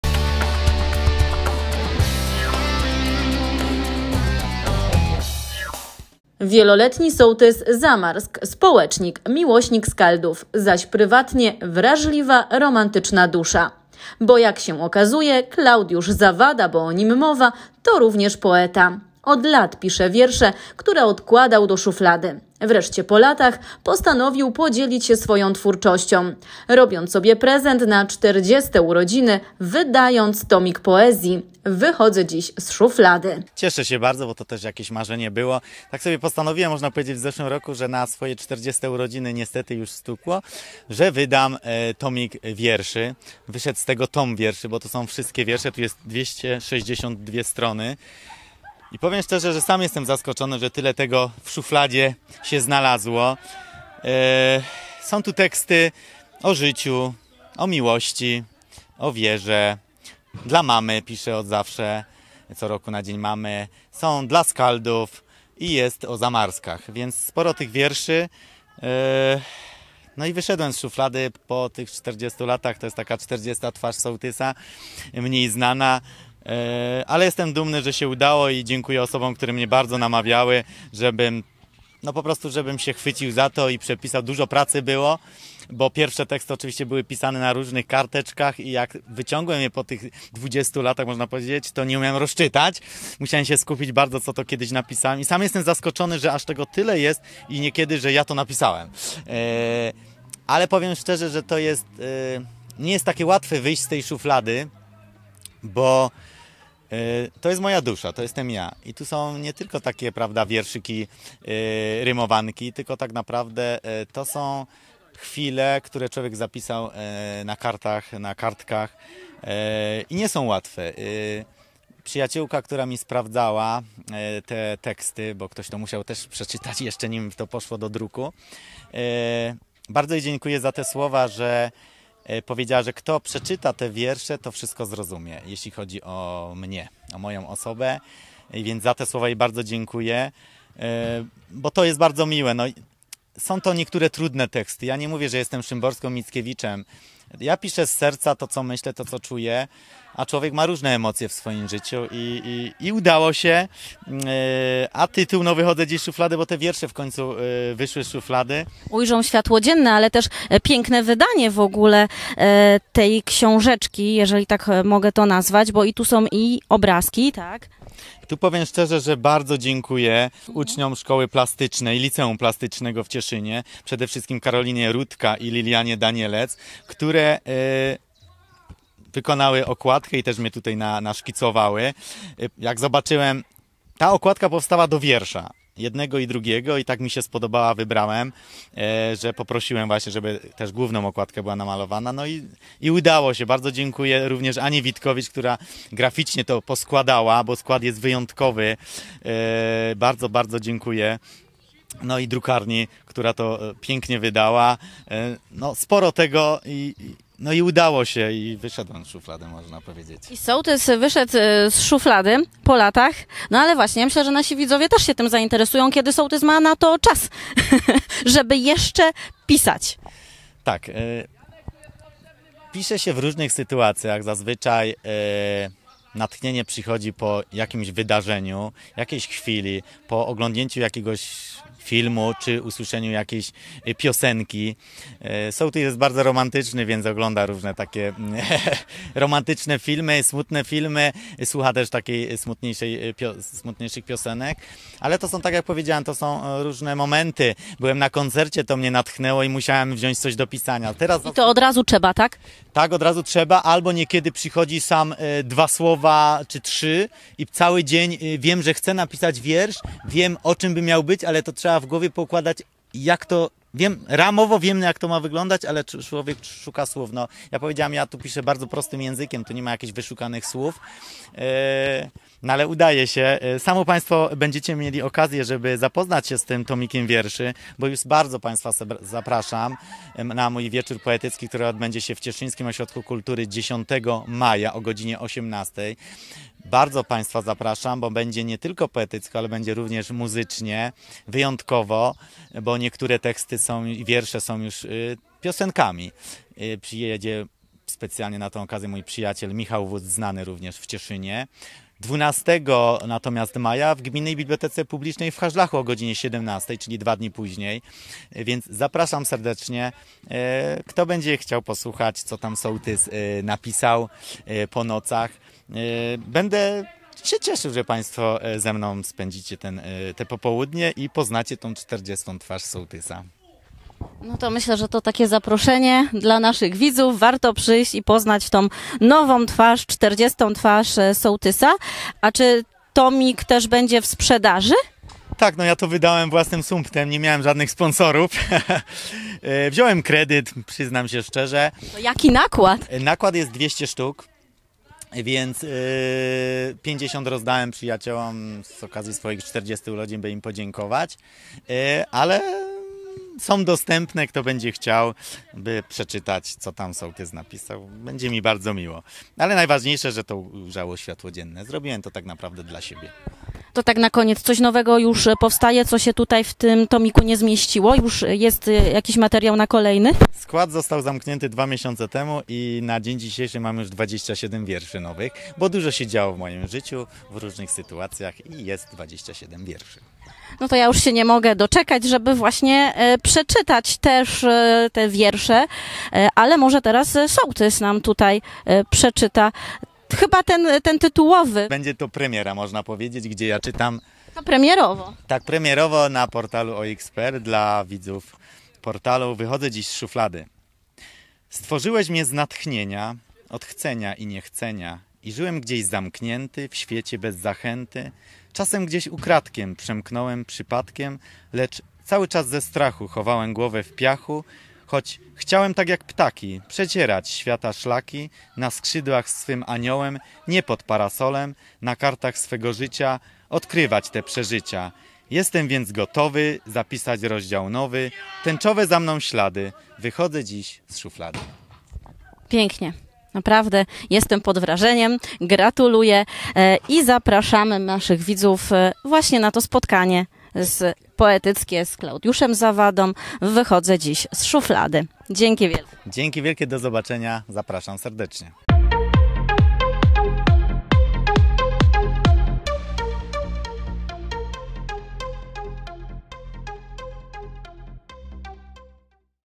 Sołtys Zamarsk wychodzi z szuflady?
Znany w regionie ze swojej aktywnej działalności na rzecz sołectwa Zamarski Klaudiusz Zawada tym razem zaprezentuje się w całkiem nowej odsłonie. Już jutro (10.05) z sołtysem Zamarsk będzie się można spotkać w Cieszynie podczas promocji jego tomiku poezji pod tytułem "Wychodzę dziś z szuflady". O tomiku, lękach, szufladzie pełnej wierszy, czasie wolnym i wenie twórczej w materiale filmowym.